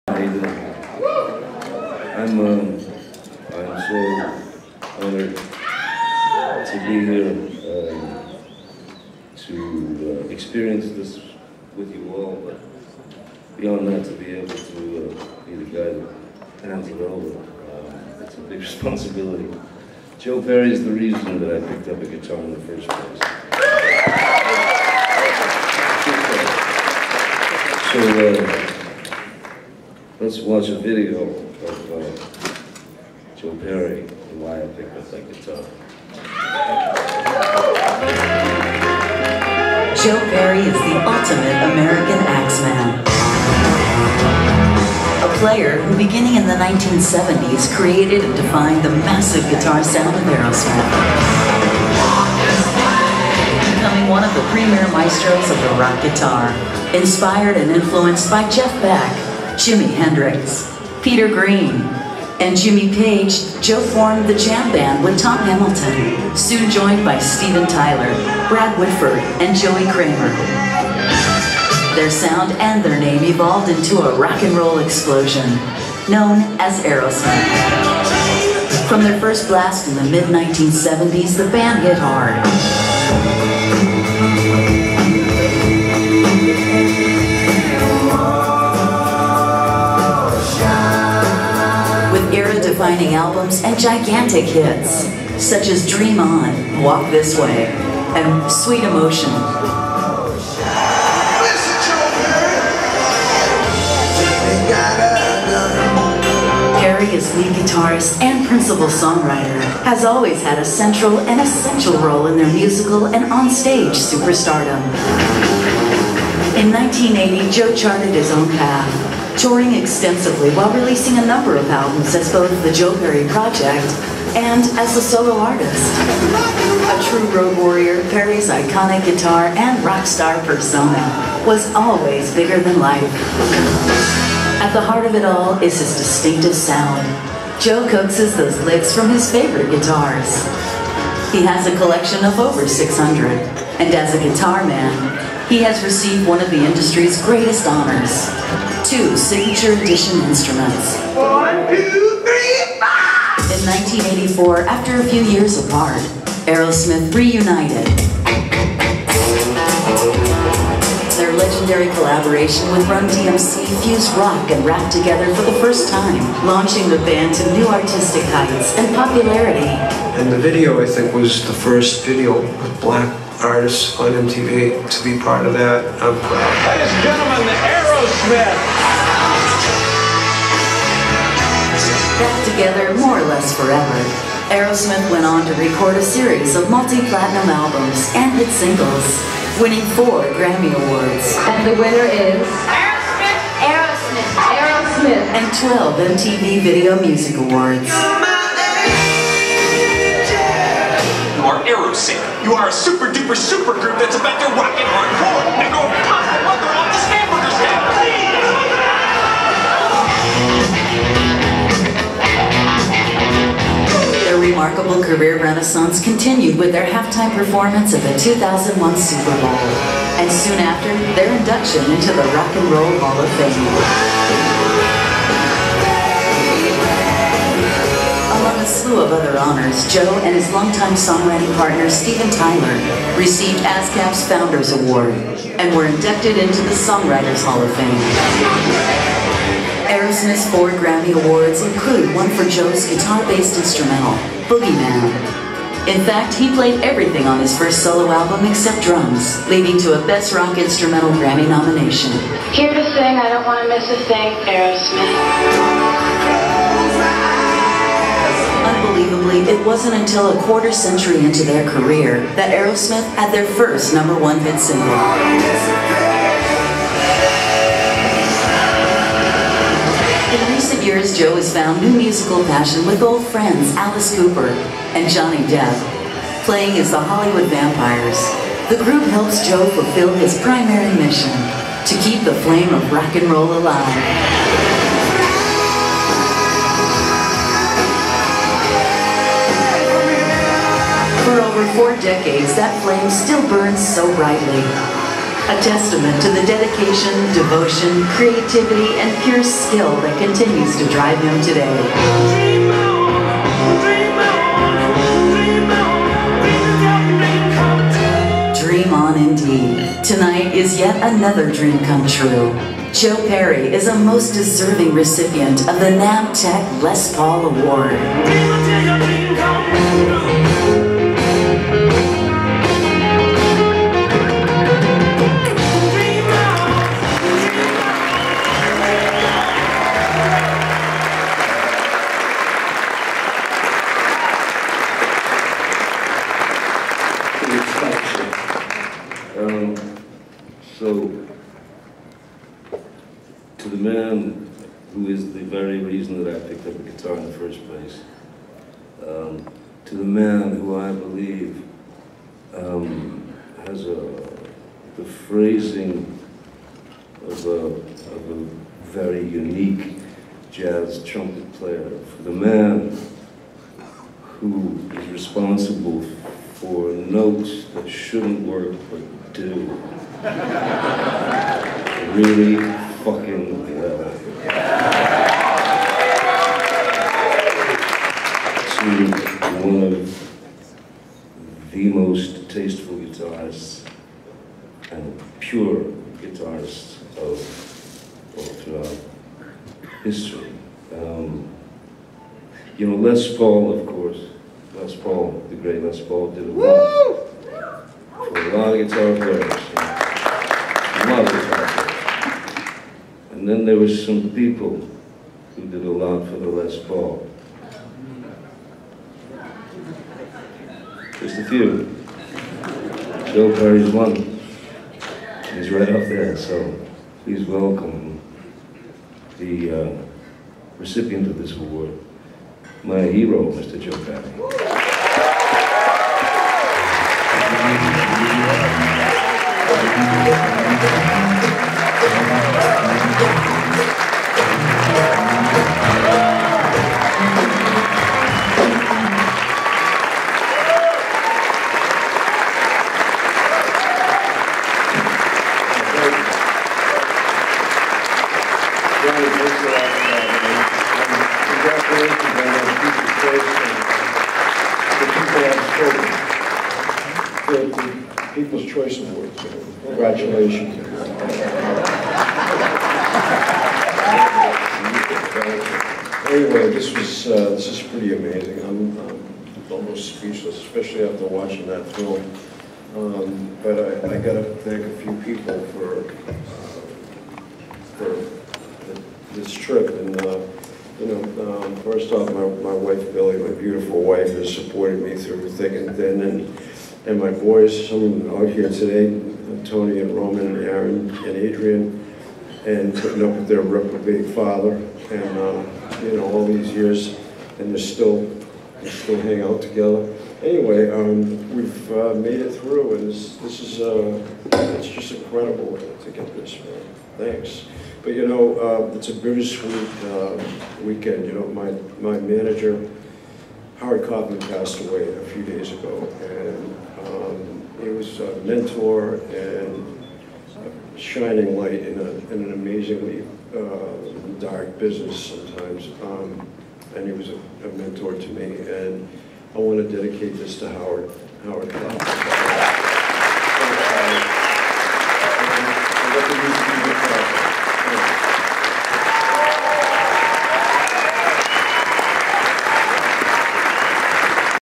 Johnny Depp presents Joe Perry NAMM's Les Paul Award (092)
Johnny Depp presents Aerosmith's Joe Perry NAMM's Les Paul Award, January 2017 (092)